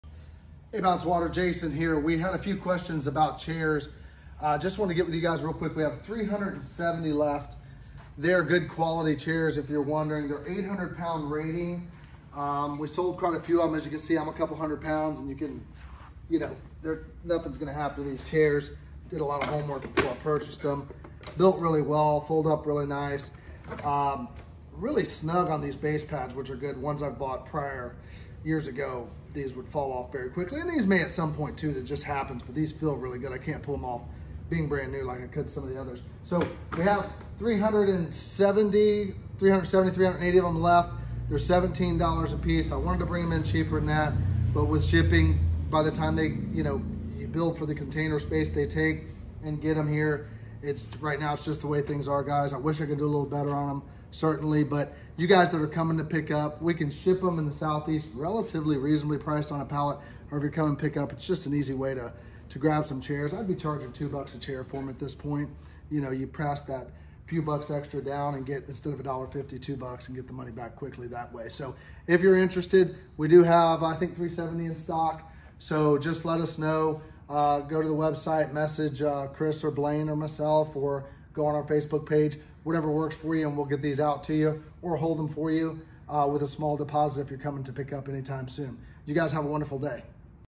Folding Chairs In Stock!
Have a seat! 🪑

We have white folding chairs IN STOCK ready to be picked up or shipped to you TODAY! 📦

Call us to place your order! 🤳